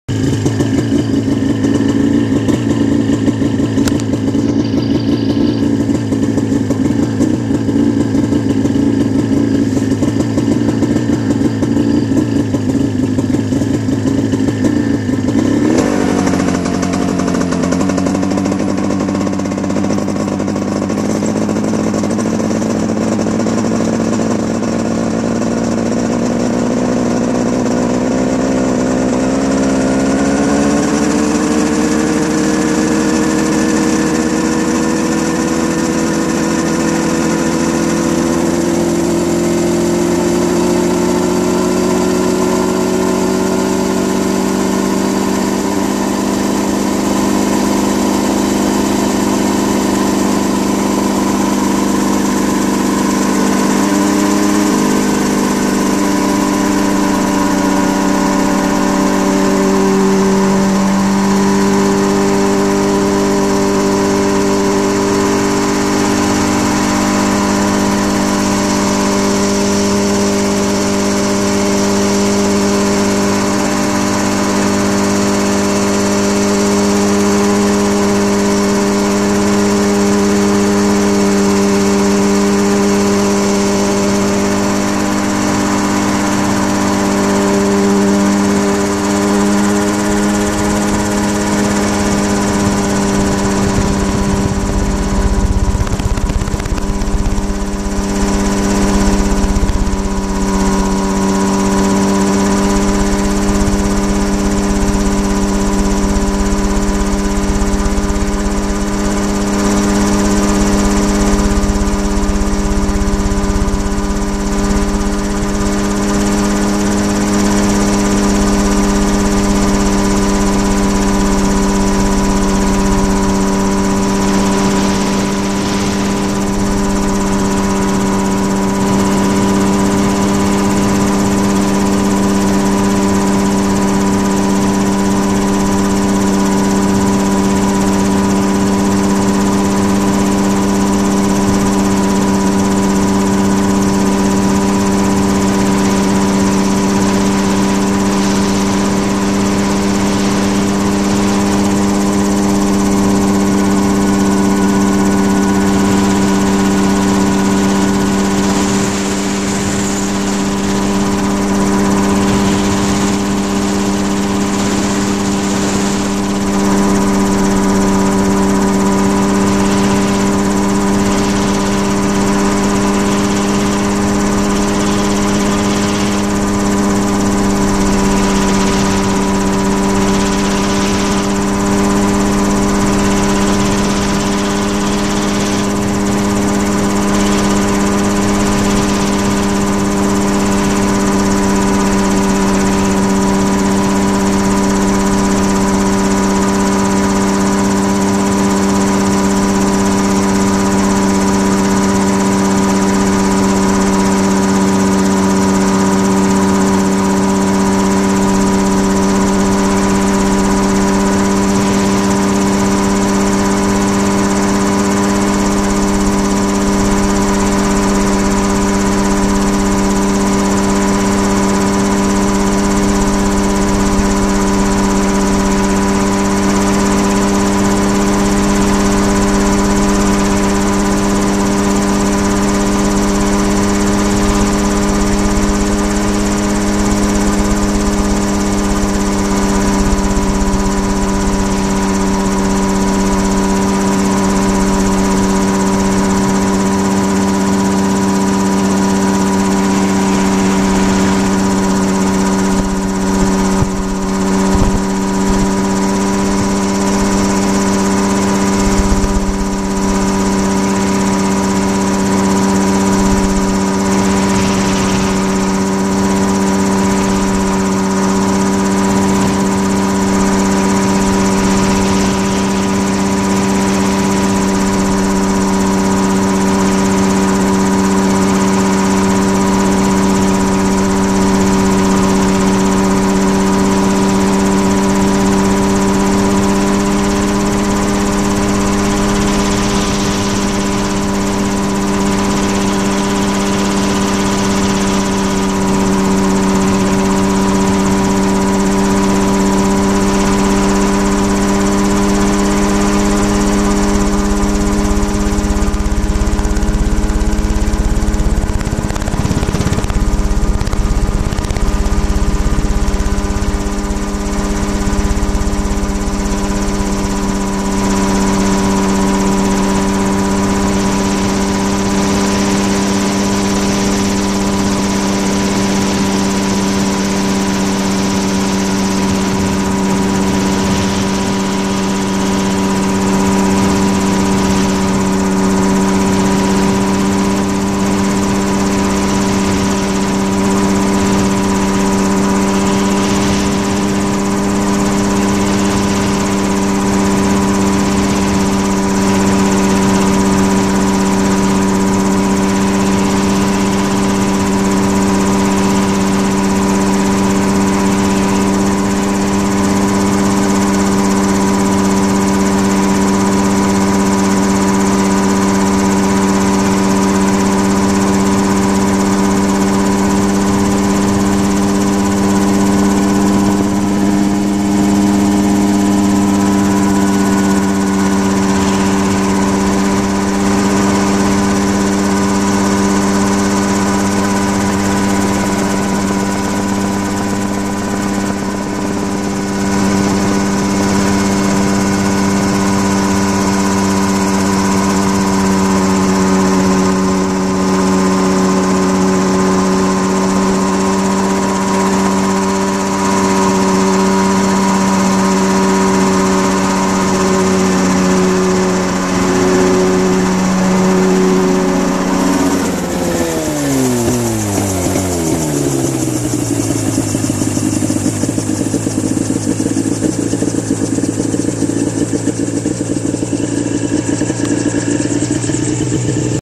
Mosquito flight to Roloff's farm, Alpaca farm, Helvetia Winery and back home
1018/09, very mild temp, light breeze.  Busy day at Matt's place.  Save the Farms event at Alpaca Farm.